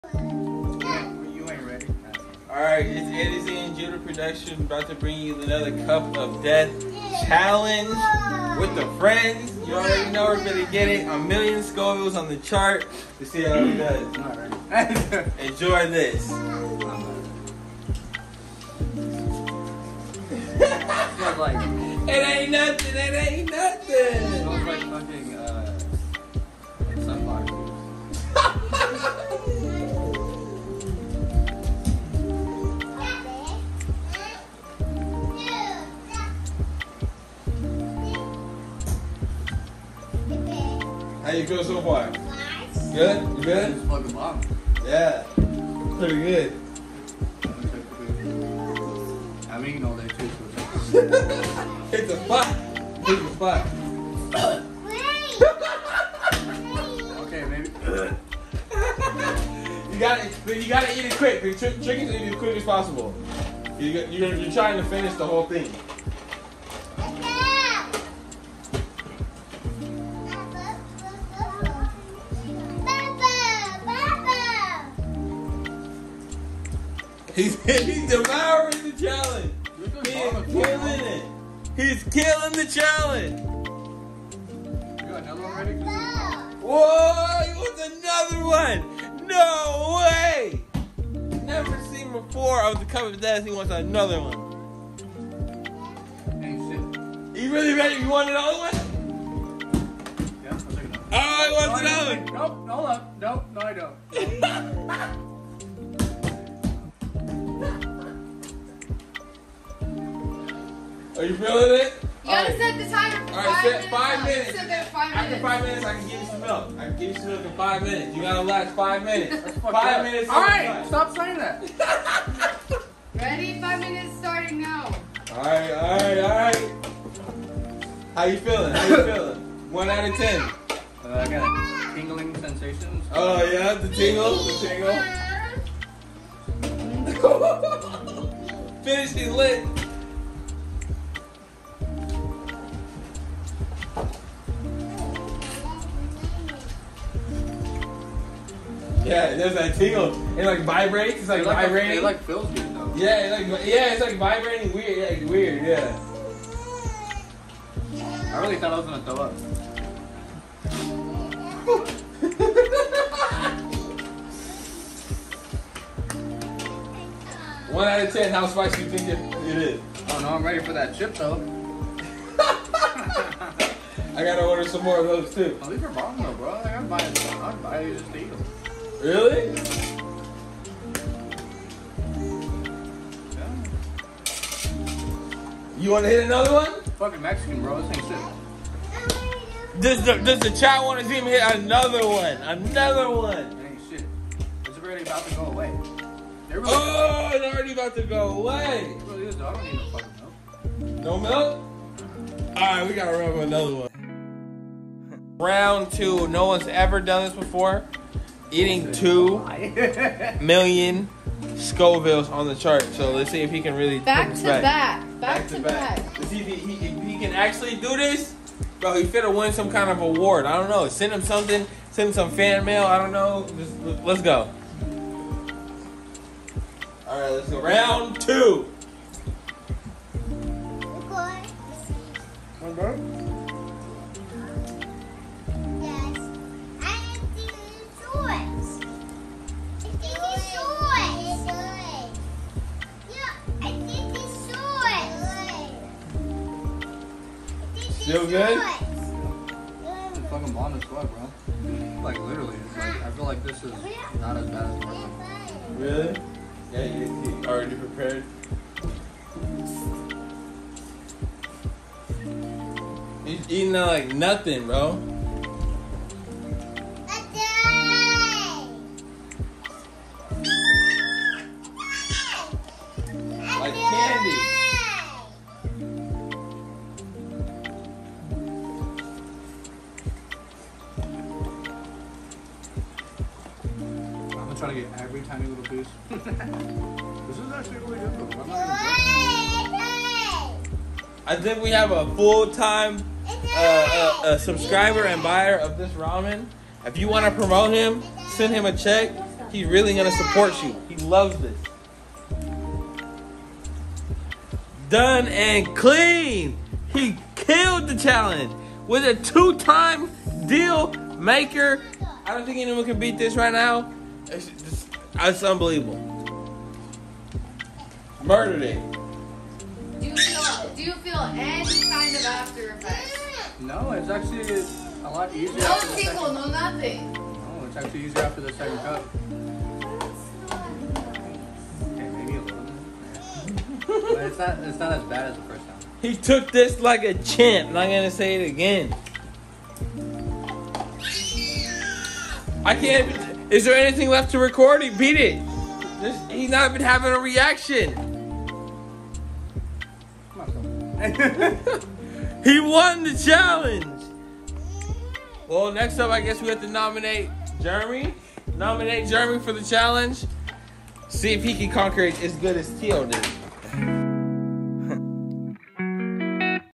Cup of death challenge 2.0
My friend teo destroys the ramen challenge